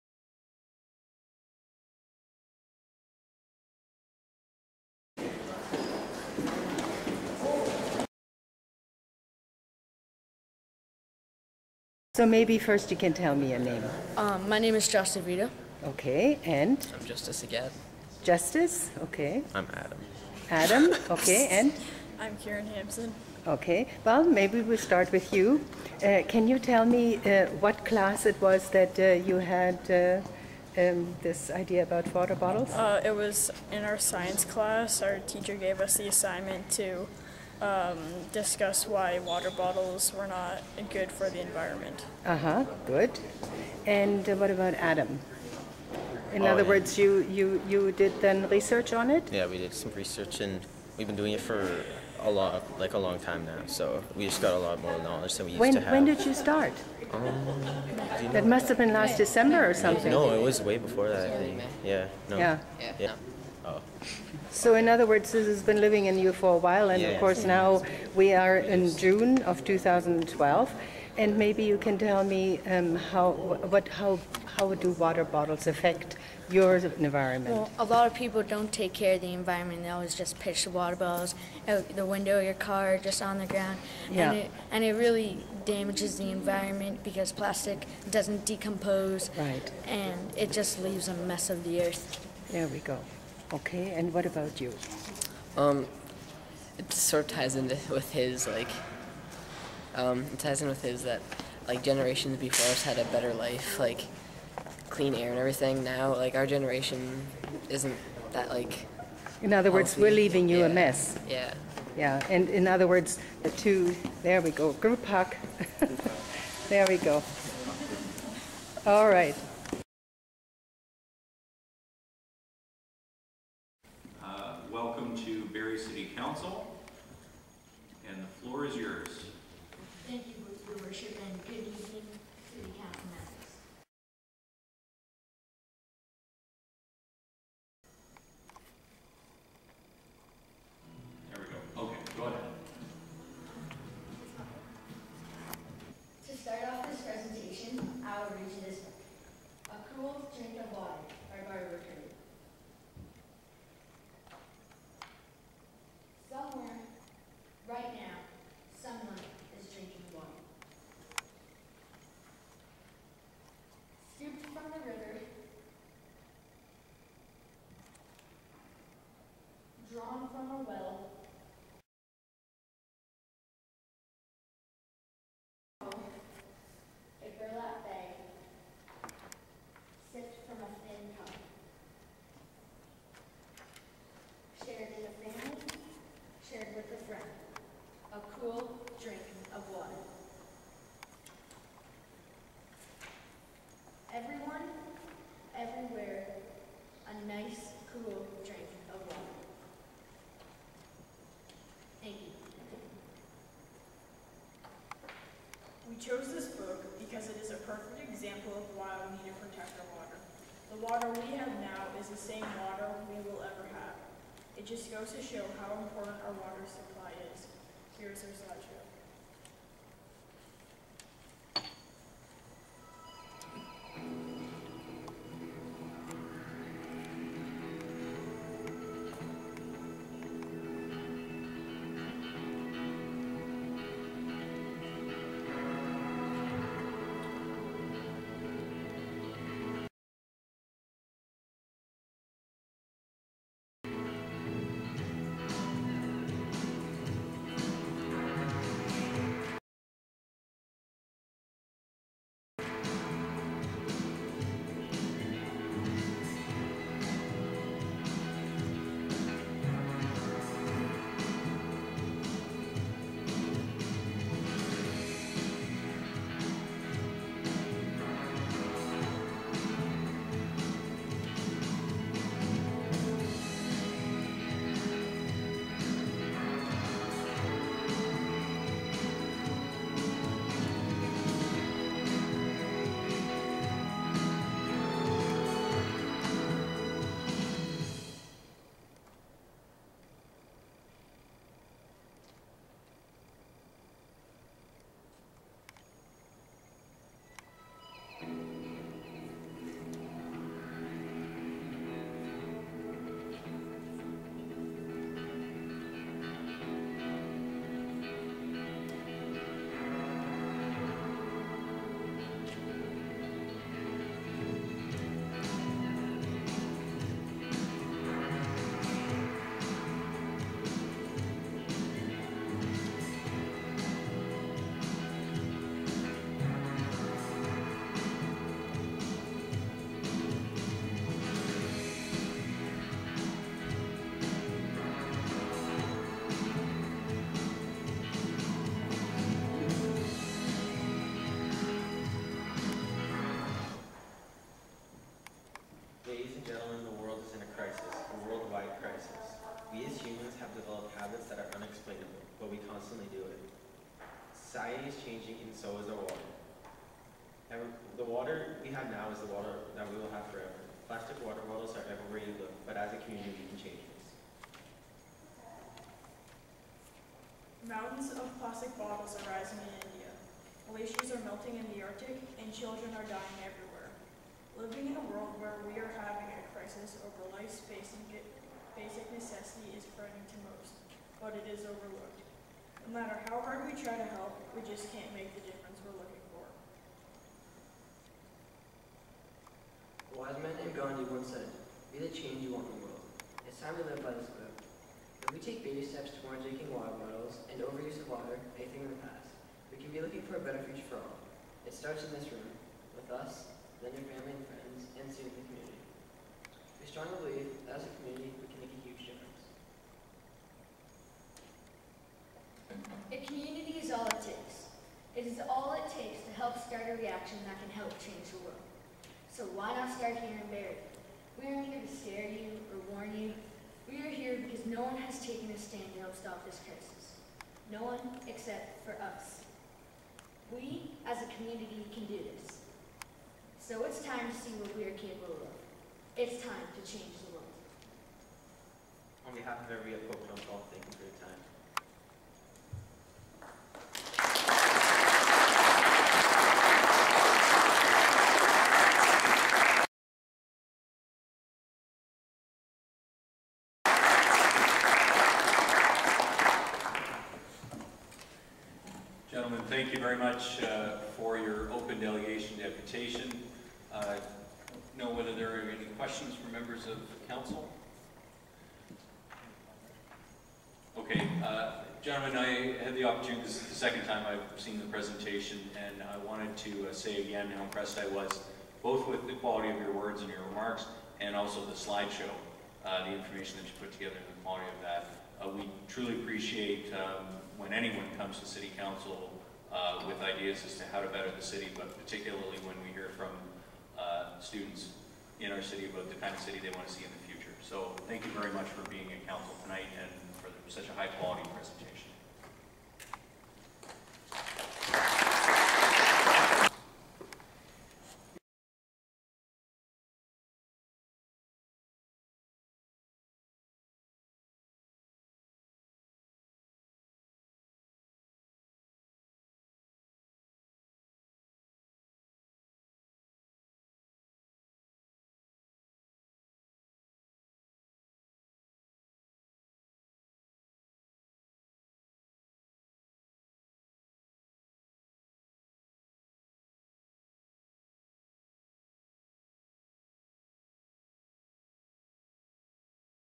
Pope John Paul II Students Blue Communities Presentation June 18, 2012
Pope John Paul II School in Barrie - four Students presented to Barrie City Council their wish for Barrie to be the next Blue Community Blue on June 19, 2012. The Council of Canadians had made their presentation to City Council in the Spring - and a motion has been referred to staff. What is a Blue Community ?
The Blue Communities Project calls on communities to adopt a water
commons framework by:
1. Recognizing water as a human right.
2. Promoting publicly financed, owned and operated water and wastewater
services.
3. Banning the sale of bottled water in public facilities and at municipal
events.